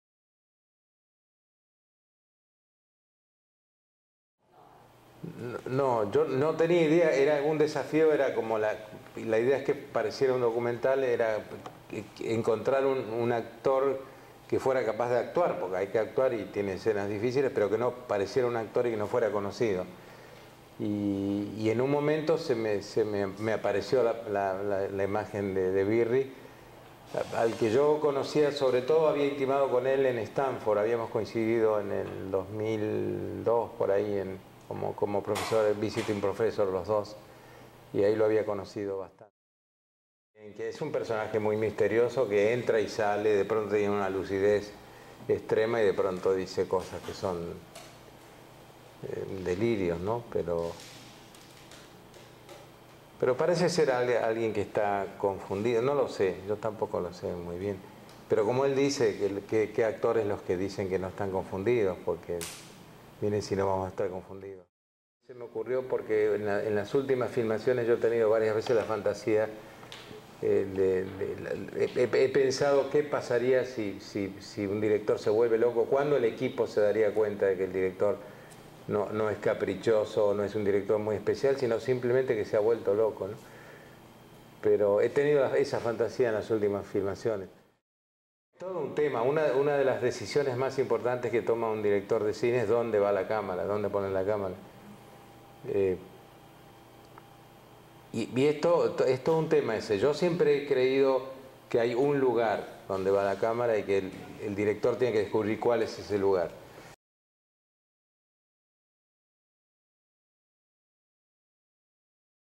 Resumen del "Paisajes Devorados" con Eliseo Subiela
El director Subiela hablando sobre su pelicula "Paisajes Devorados" despues de una presentación en UC Berkeley el 16 de octubre del 2012.

En Español.